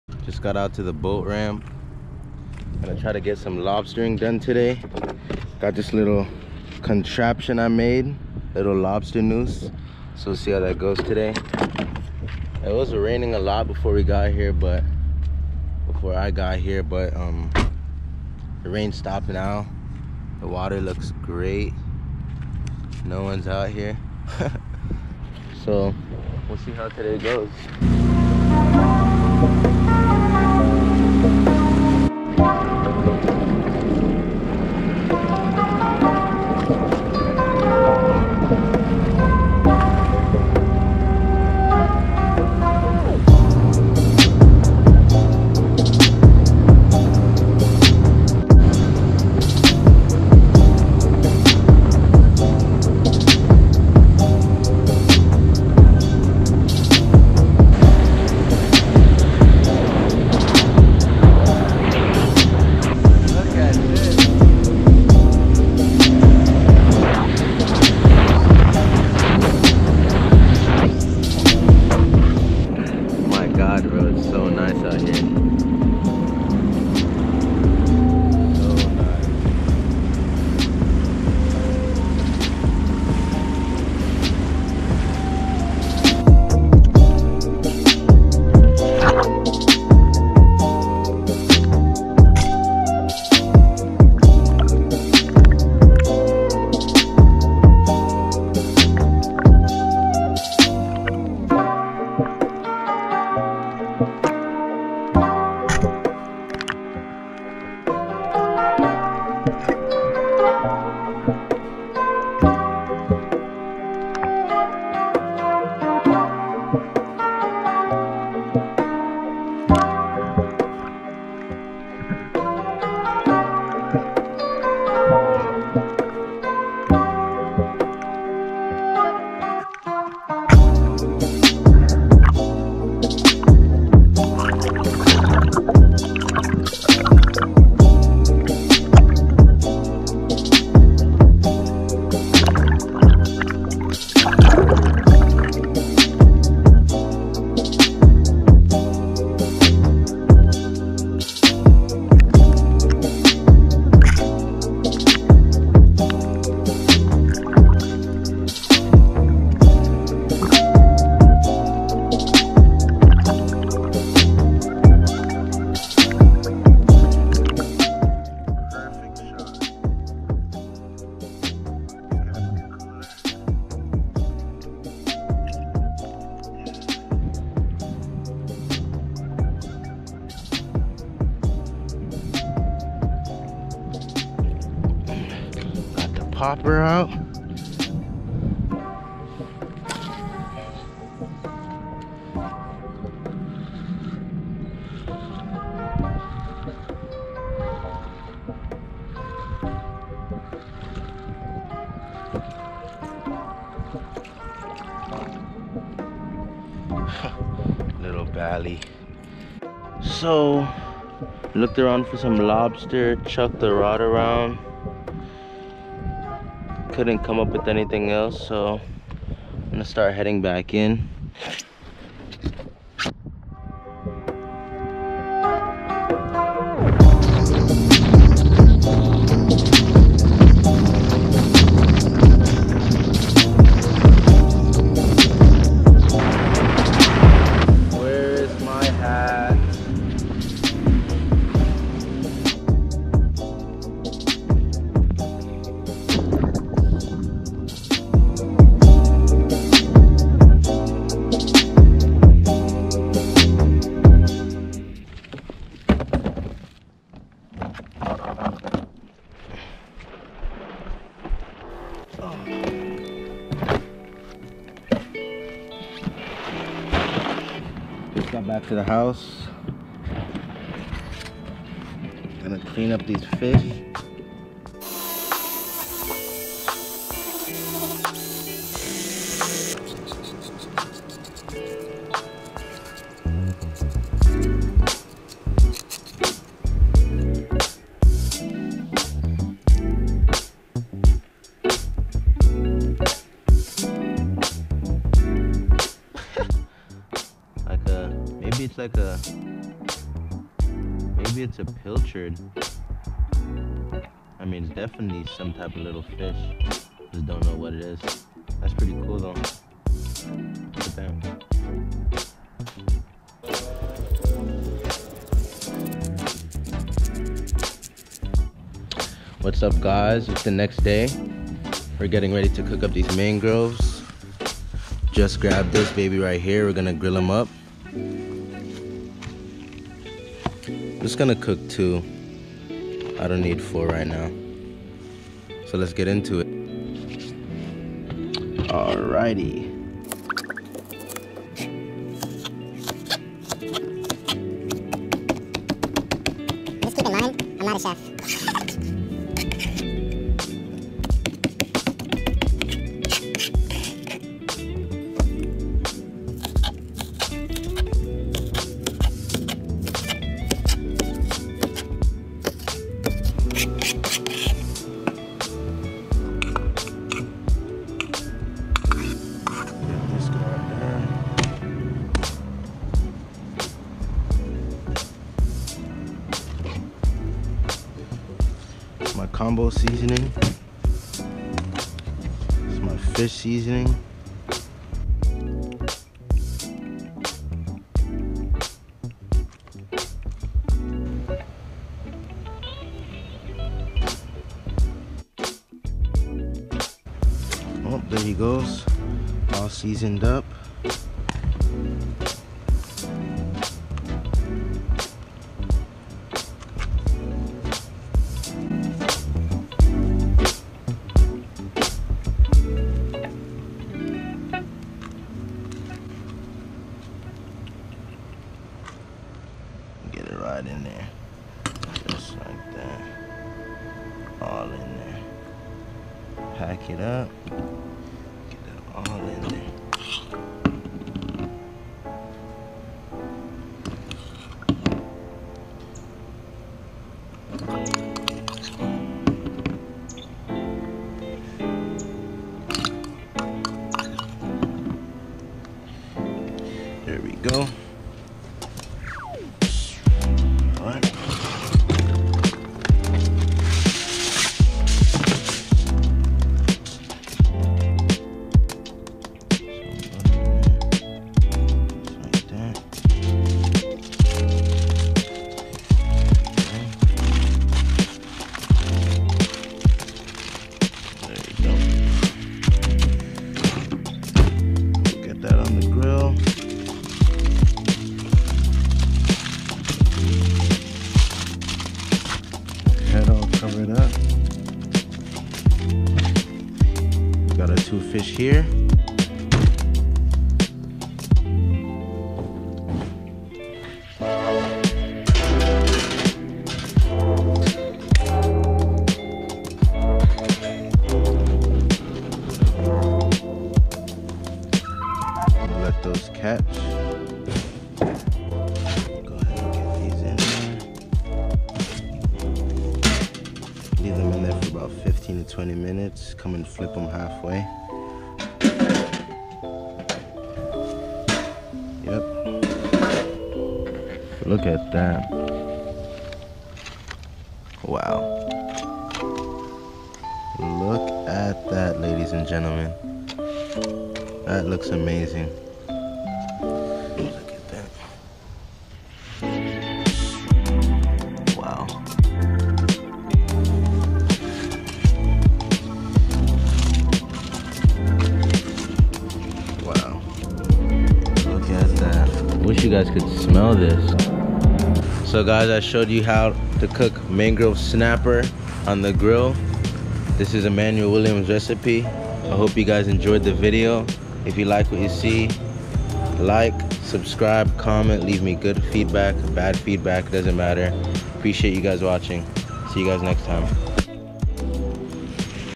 ISLAND SPEARFISHING | MANGROVE SNAPPER CATCH AND COOK
Solo send on the ski out to one of my favorite islands to grab dinner, Enjoy!